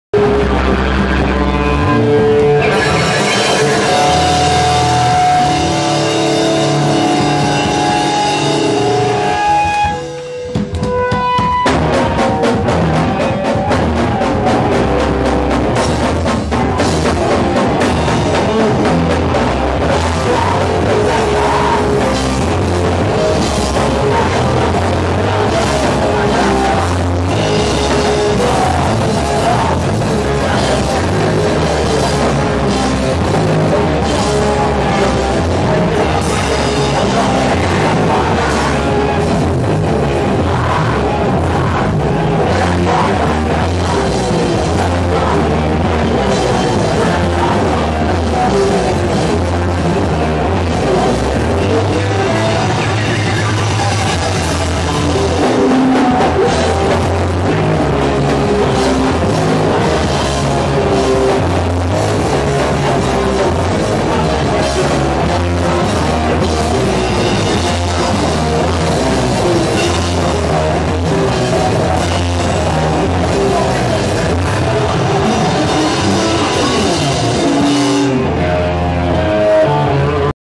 Insubordinate @ the Viaduct - December 16 2007 pt.2
Insubordinate - pt. 2 December 16th 2007, at the Viaduct in Tacoma. Real shizz - hit up the gay bar next door. It's real chill.